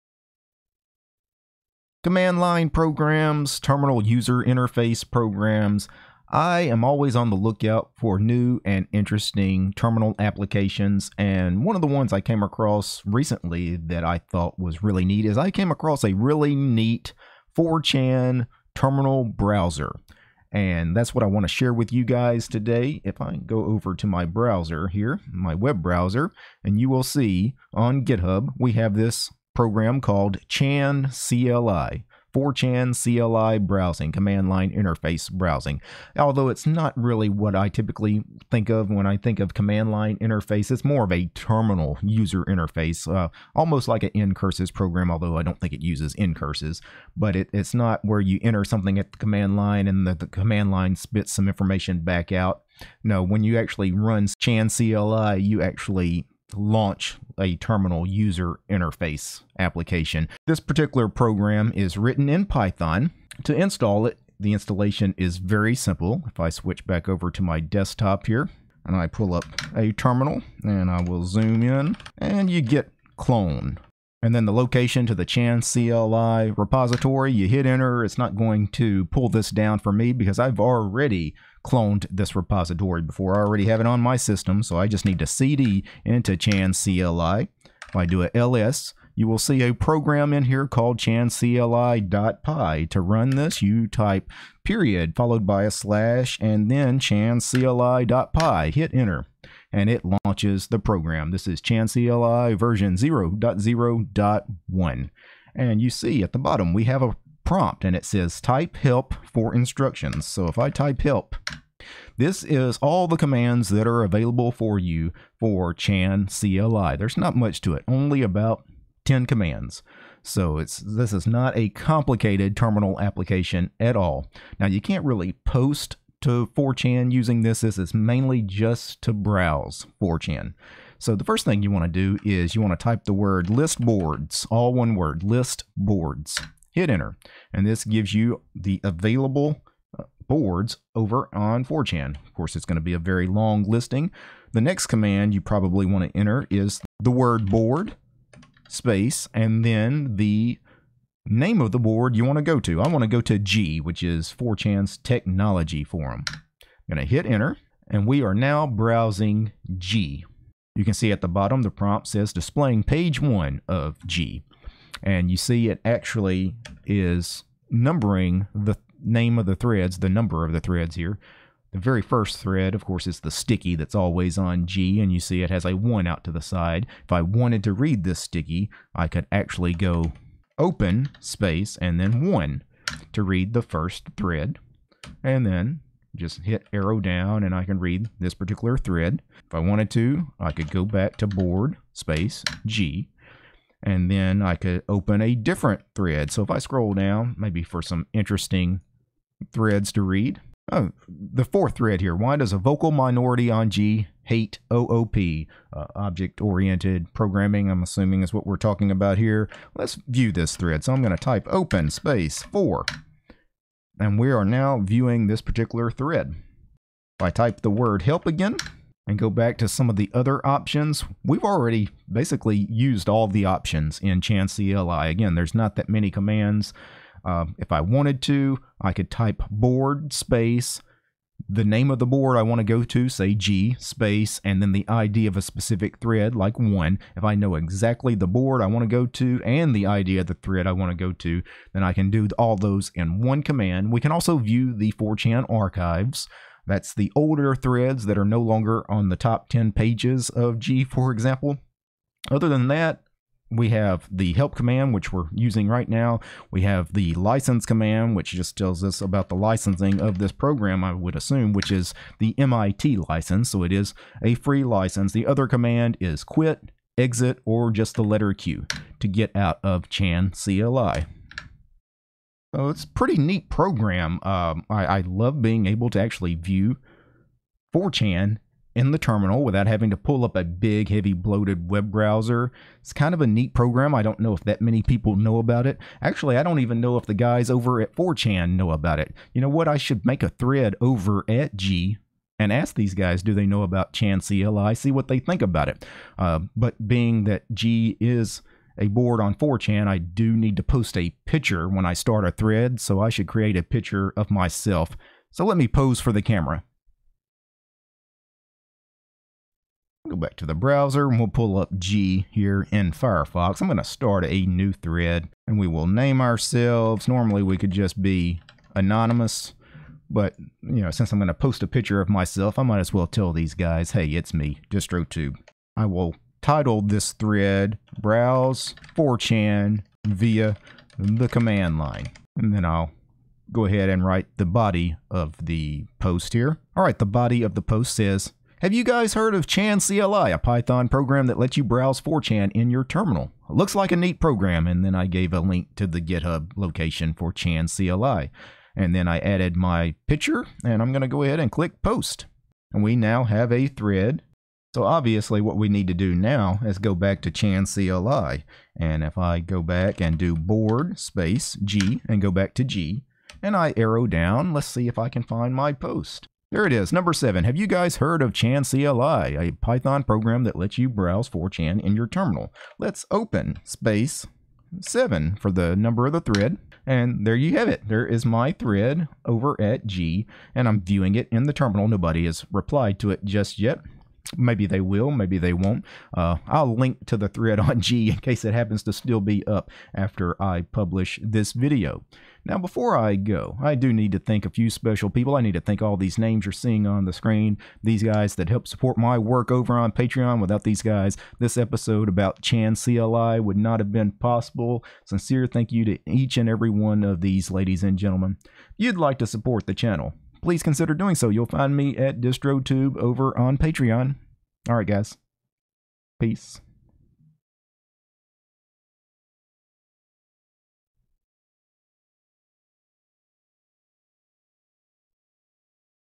4Chan At The Command Line
Today, I'm sharing with you guys a neat terminal-based 4chan browser called chancli.  It has a simple interface that intuitive and easy-to-use.